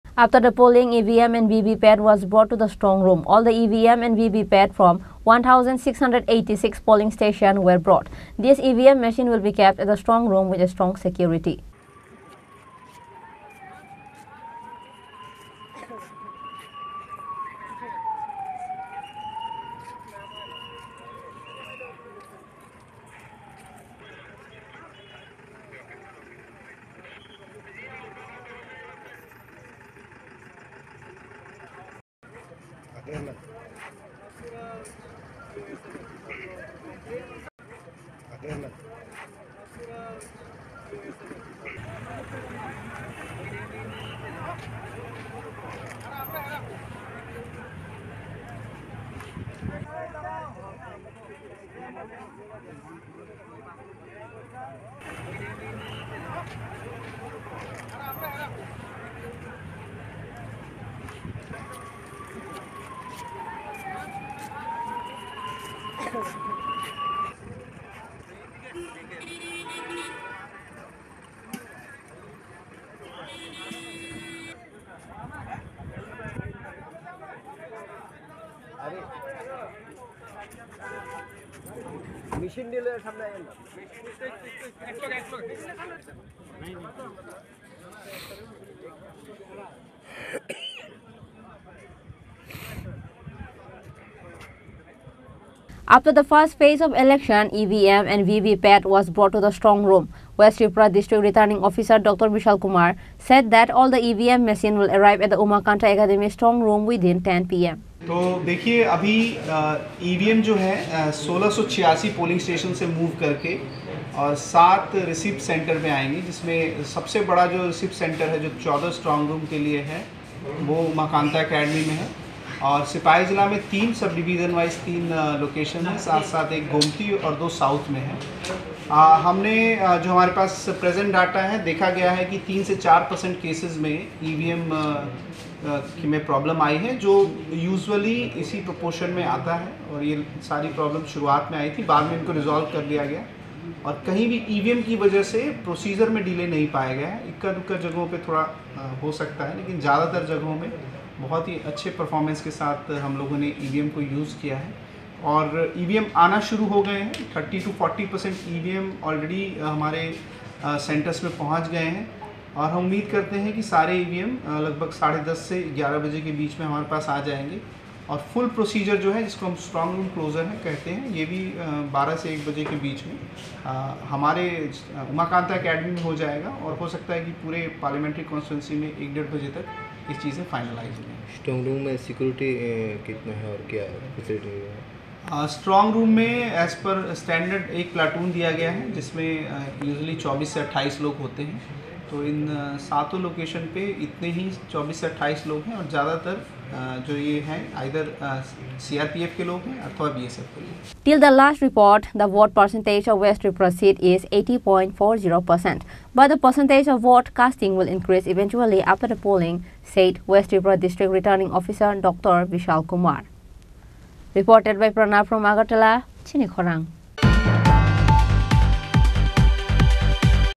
All the EVM and VV pad from 1686 Polling Station were brought at strong room after the election
🟢After the Polling, EVM and VV pad was brought to the Strong room. All the EVM and VV pad from 1686 Polling Station were brought. This EVM machine will be kept at the Strong room with a strong security.